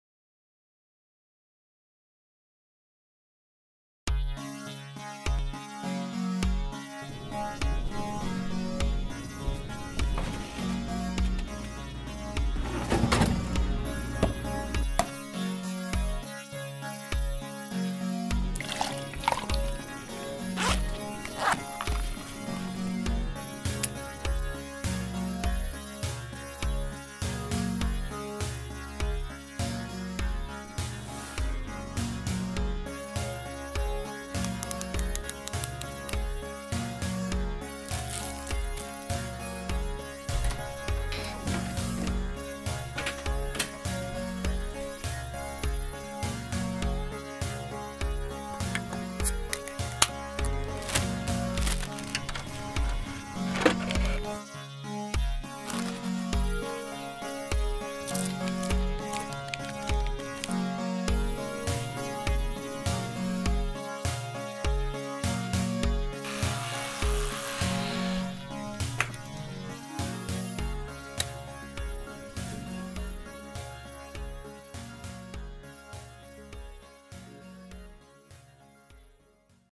A Day in the Life of a Diabetic -HD-
This video was created for an audio and video class.  The assignment was to create a short 60s film with the subject "A day in the life."  I chose to highlight the life of my diabetic brother.